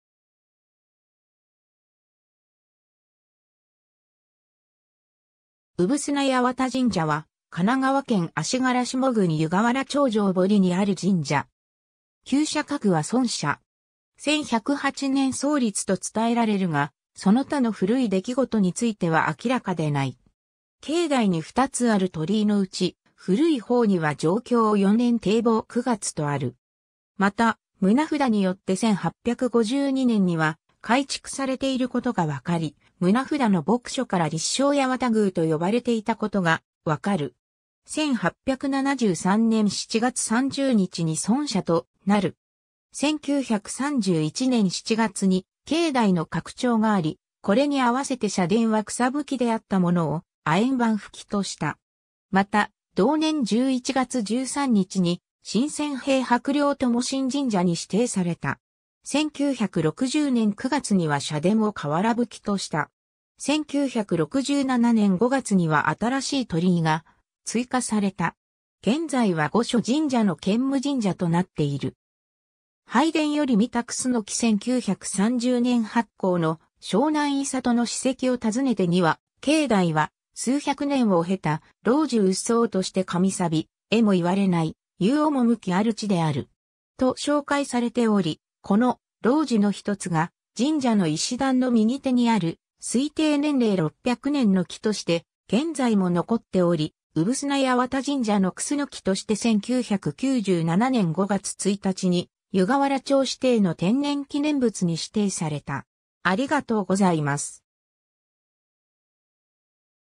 産土八幡神社
産土八幡神社（うぶすなはちまんじんじゃ）は、神奈川県足柄下郡湯河原町城堀にある神社。
 1108年（天仁元年）創立と伝えられるがその他の古い出来事については明らかでない。
境内に二つある鳥居のうち、古い方には貞享4年（1687年）丁卯9月とある。
また、棟札によって1852年（嘉永5年） には改築されていることがわかり、棟札の墨書から立正八幡宮と呼ばれていたことがわかる。
1873年（明治6年）7月30日に村社となる。
1931年（昭和6年）7月に境内の拡張があり、これにあわせて社殿は草葺であったものを亜鉛版葺とした。
また、同年11月13日に神饌幣帛料供進神社に指定された。
1960年（昭和35年）9月には社殿を瓦葺とした。
1967年（昭和42年）5月には新しい鳥居が追加された。
現在は五所神社の兼務神社となっている。
 拝殿より見た楠 1930年発行の『湘南五郷の史蹟を訪ねて』には「境内は数百年を経た老樹鬱蒼として神さび、えもいはれない幽趣ある地である。」と紹介されており、この「老樹」のひとつが神社の石段の右手にある推定年齢600年の木として現在も残っており、「産土八幡神社の楠」として1997年（平成9年）5月1日に湯河原町指定の天然記念物に指定された。